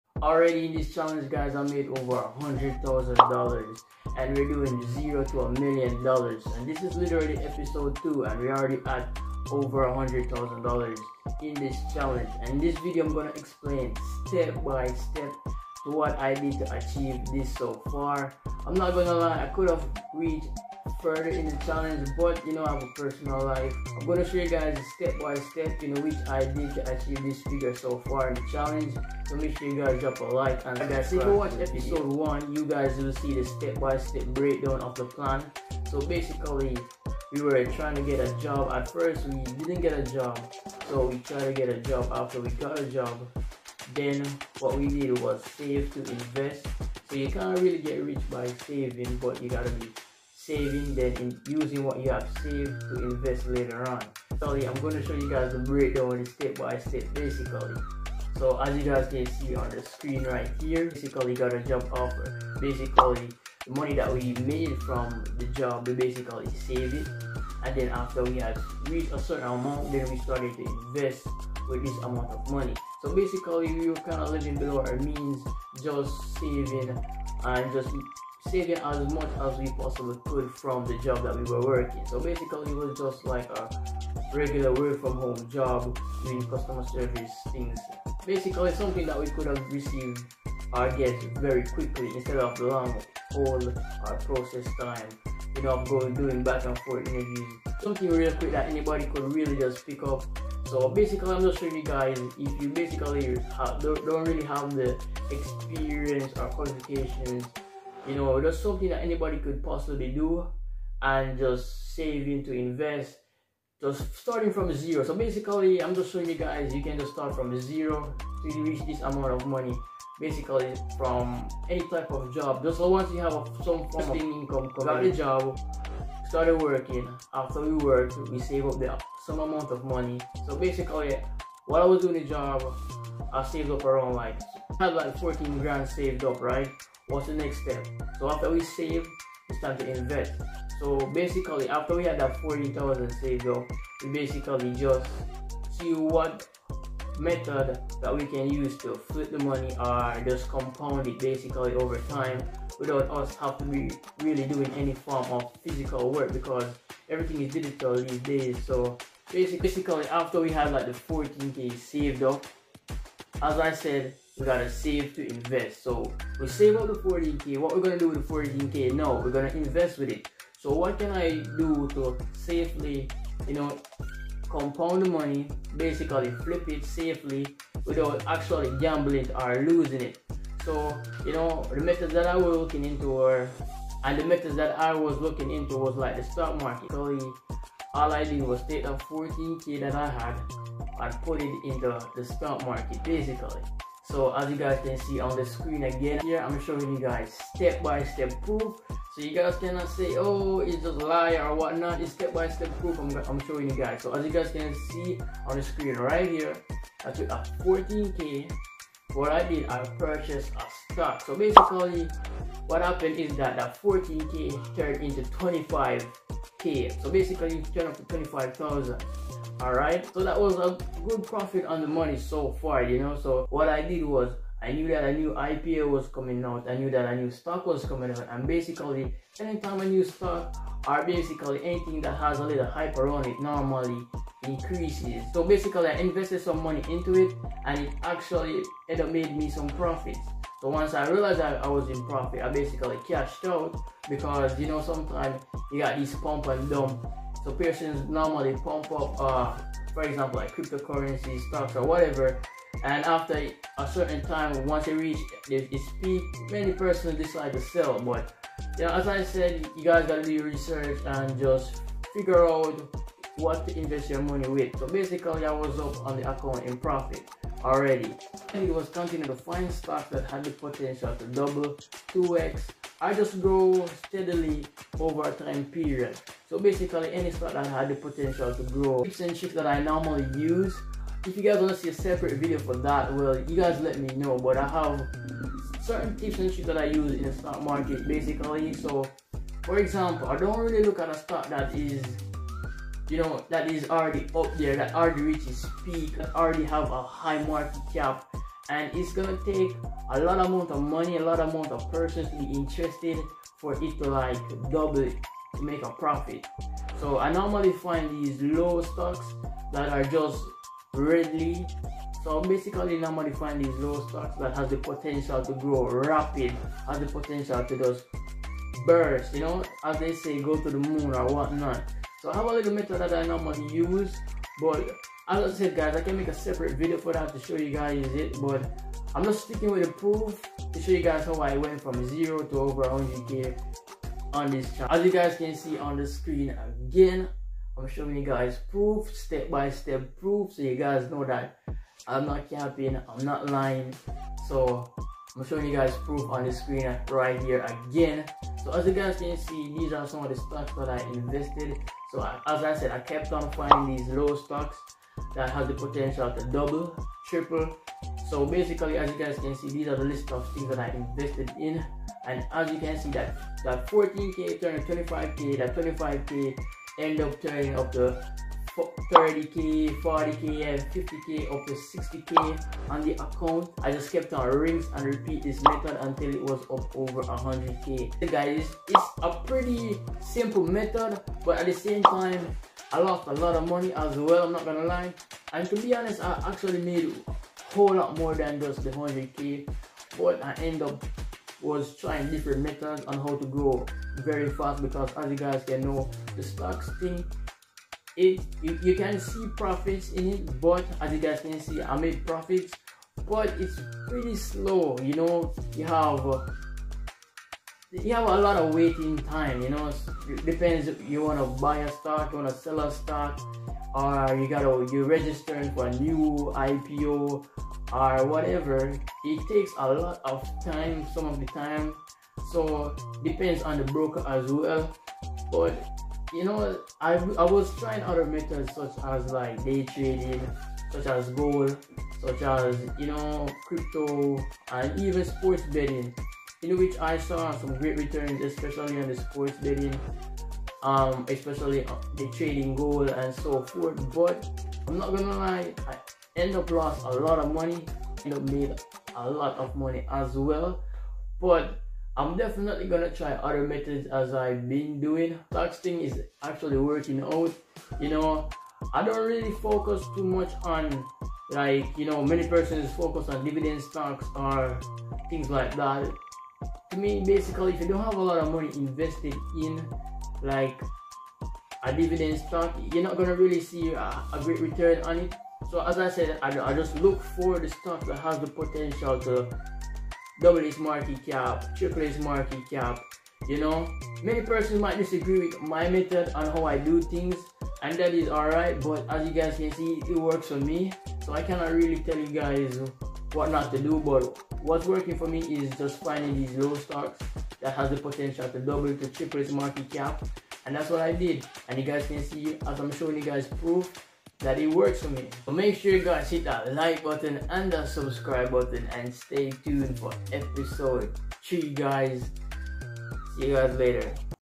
I Turned $0 into $100,000 ($0 to 1million ep.2)
I Turned $0 into $100,000 in the stock market (zero to million ep.2)